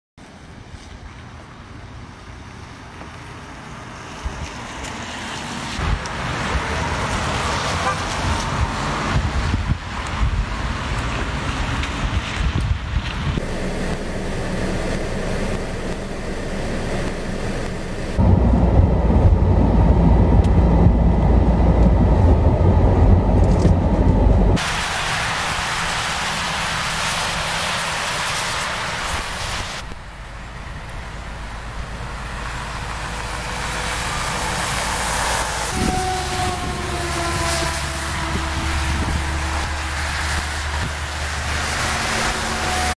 Snow Falls in Southern Pines
Moore County experienced its second measurable snow event of 2018 on March 12. (Video by Jaymie Baxley/The Pilot)